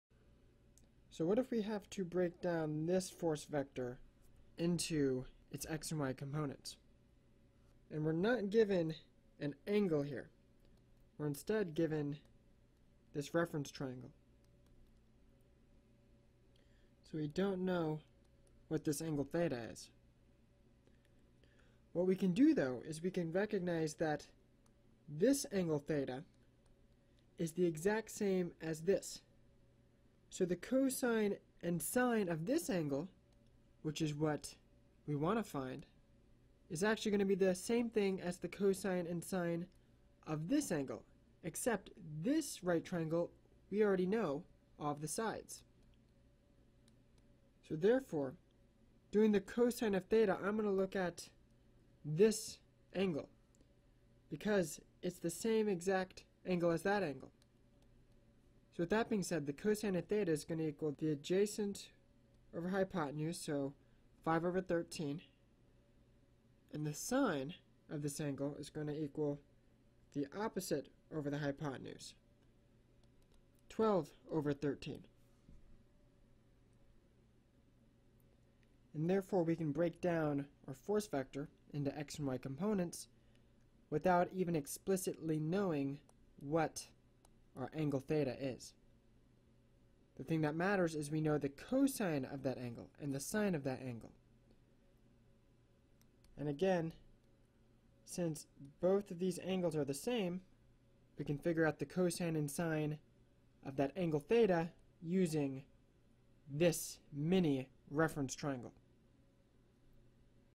Statics: Using a Reference Triangle to find Vector components
Sometimes, the direction of an angle won't be given as an angle, rather, it will be given using a "reference triangle" to establish the direction. Here, we use this reference triangle to find the x and y components of a Force vector